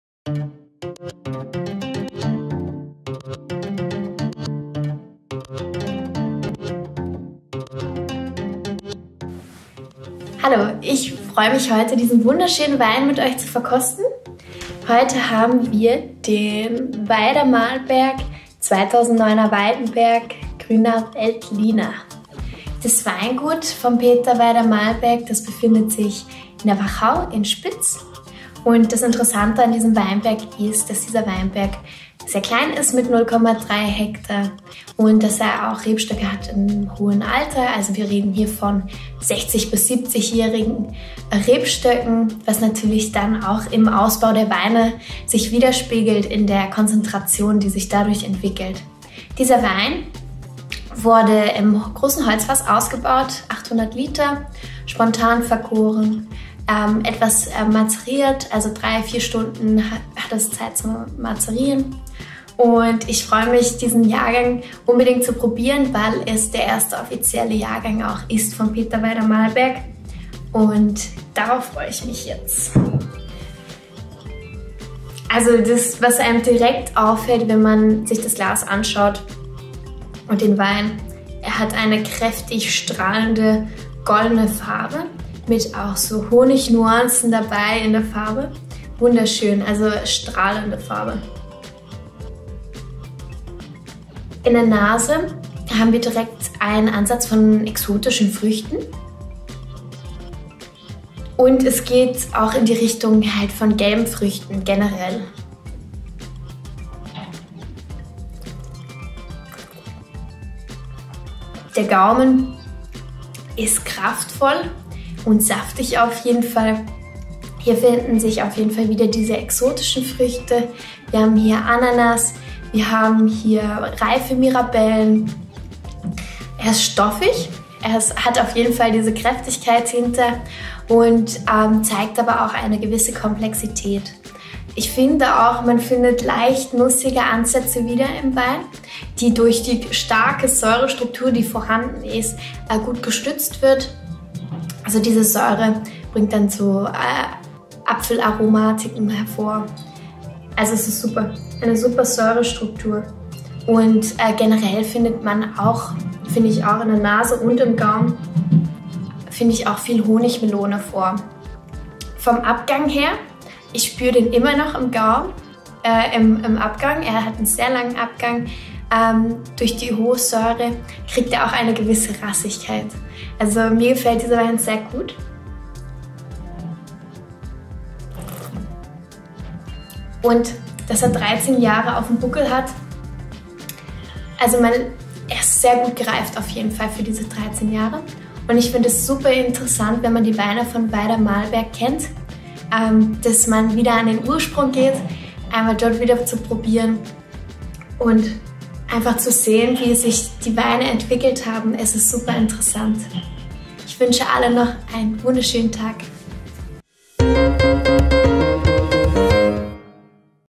trinkreif Verkostungsnotizen Veyder-Malberg Grüner Veltliner Weitenberg 2009
Melissa Lohmeyer verkostet Peter Veyder-Malberg Grüner Veltliner Weitenberg 2009 und hält ihre Eindrücke in dieser Verkostungsnotiz fest